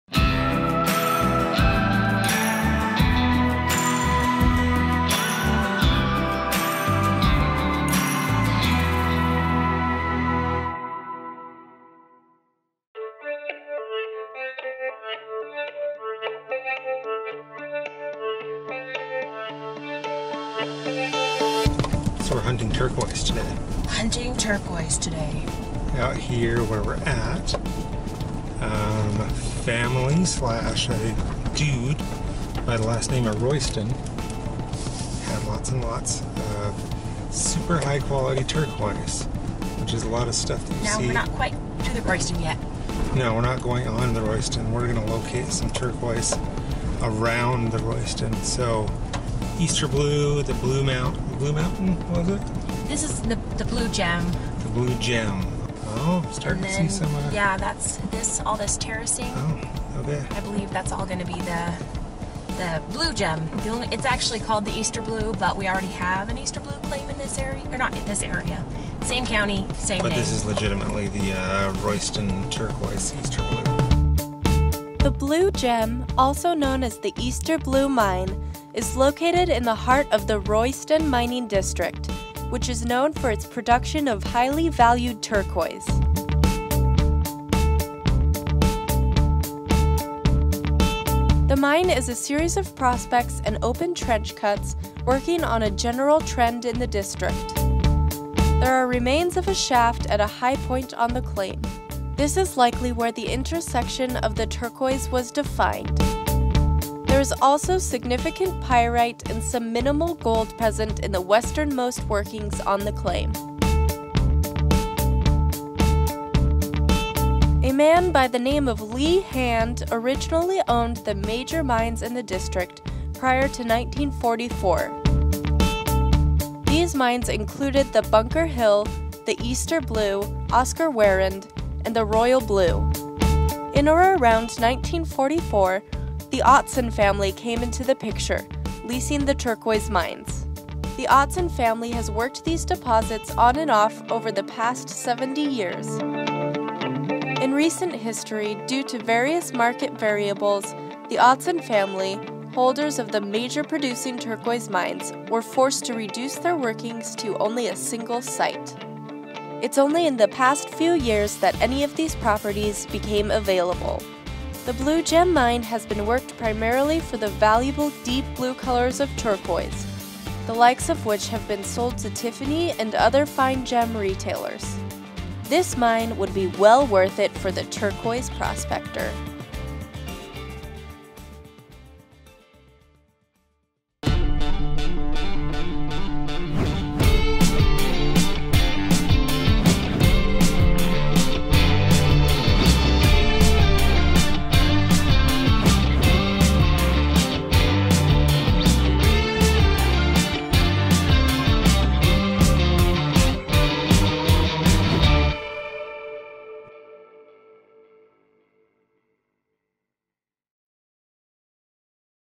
Blue Gem Turquoise Mining Claim - Nevada - 2017
The Blue Gem, also known as the Easter Blue Mine, is located in the heart of the Royston Mining District, which is known for its production of highly valued turquoise.